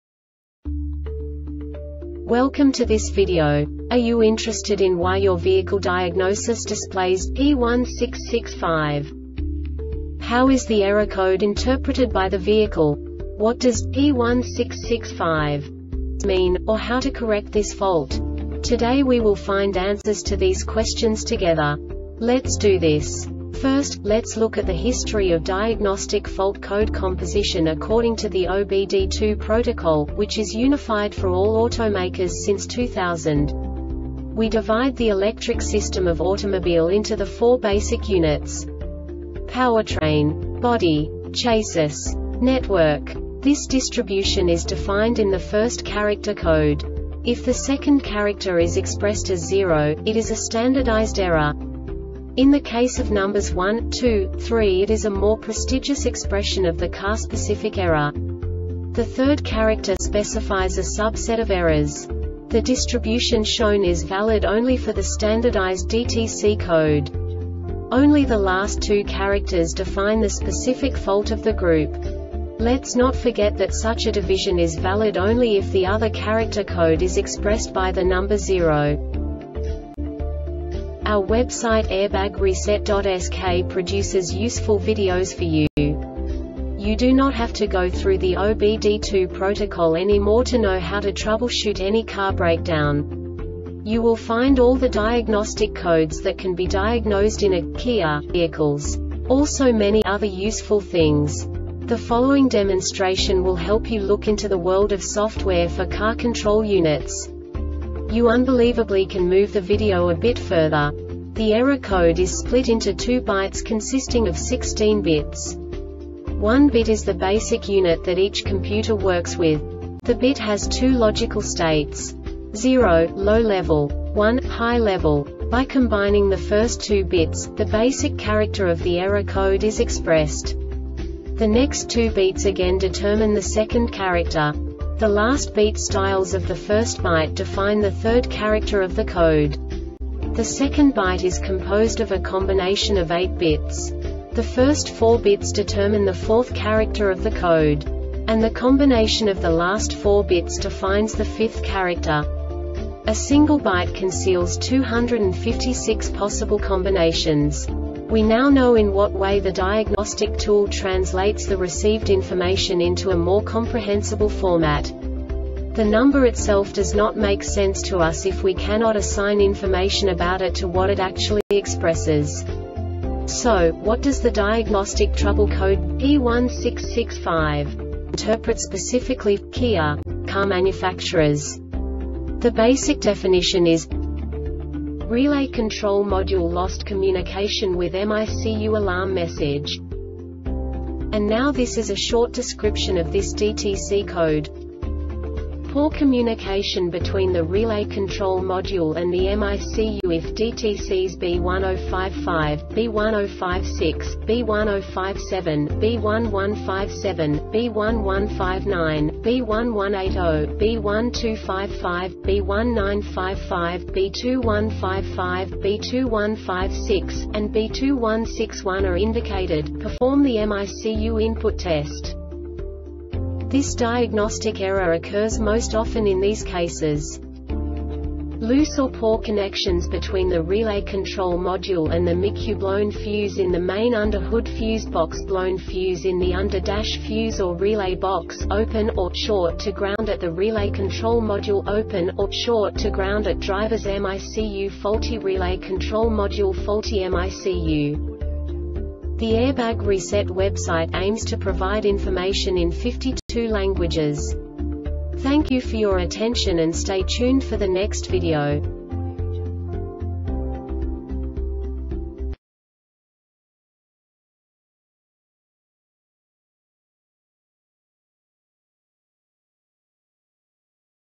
DTC KIA P1665 Short Explanation
Contents:
0:21 Basic DTC analysis according to OBD2 protocol standard.
1:48 Insight into programming
2:58 A diagnostically understandable description of the Diagnostic Trouble Code
3:05 Basic error definition

Make:
Kia
Code:
P1665
Definition:
Relay Control Module Lost Communication with MICU (ALARM Message)
Description:
Poor communication between the Relay Control Module and the MICU If DTCs B1055, B1056, B1057, B1157, B1159, B1180, B1255, B1955, B2155, B2156, and B2161 are indicated,perform the MICU input test.
Cause:
 Loose or poor connections between the Relay Control Module and the MICUBlown fuse in the main under-hood fuse box Blown fuse in the under-dash fuse or relay box "Open" or "Short" to ground at the Relay Control Module "Open" or "Short" to ground at drivers MICUFaulty Relay Control Module Faulty MICU